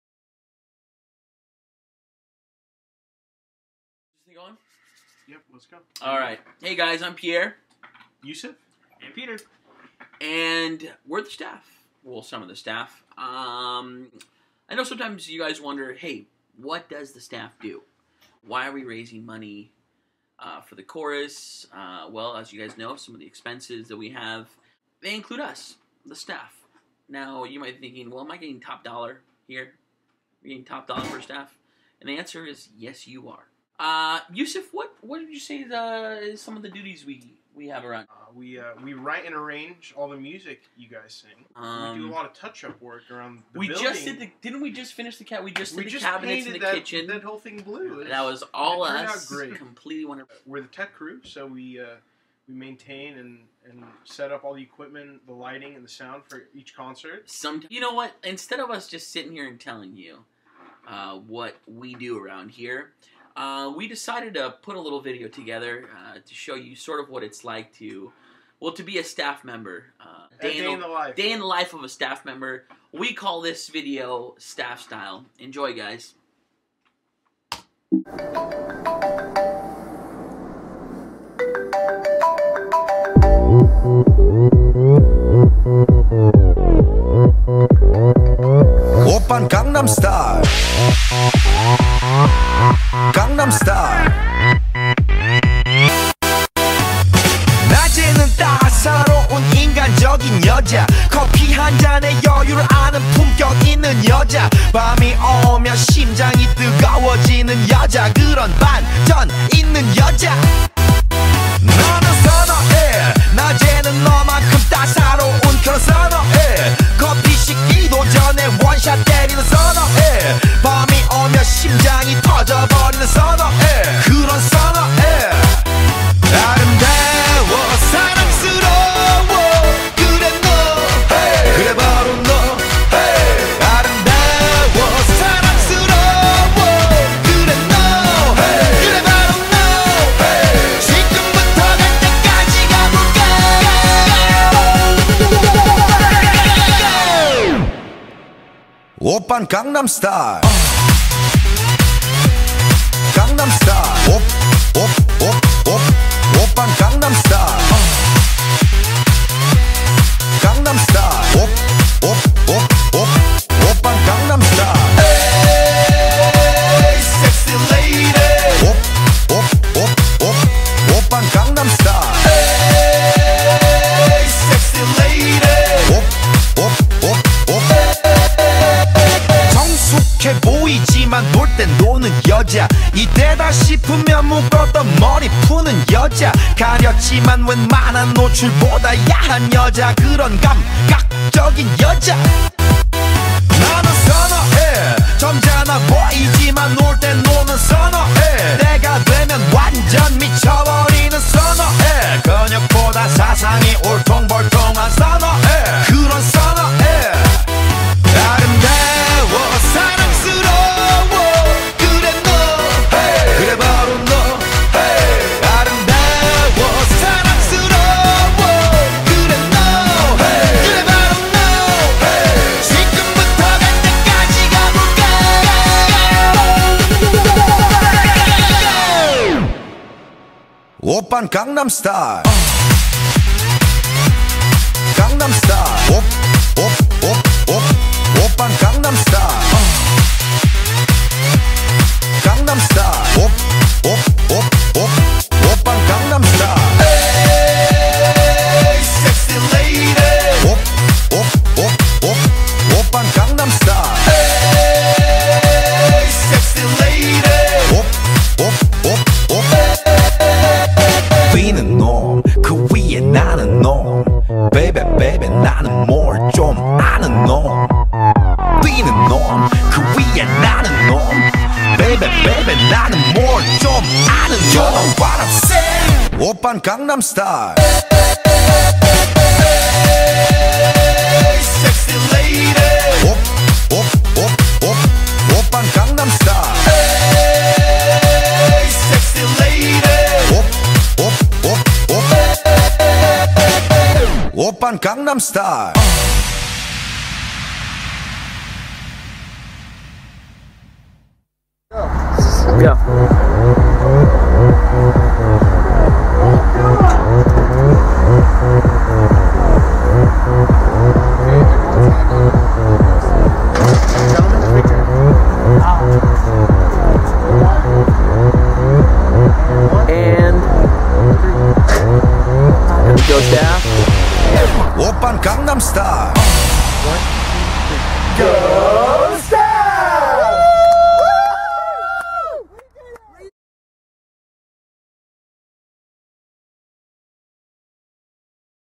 Staff Style
The AABC's staff made a video for our annual Jog-A-Thon to show the boys what a typical day at work is like when you are employed at the AABC.

Like the AABC?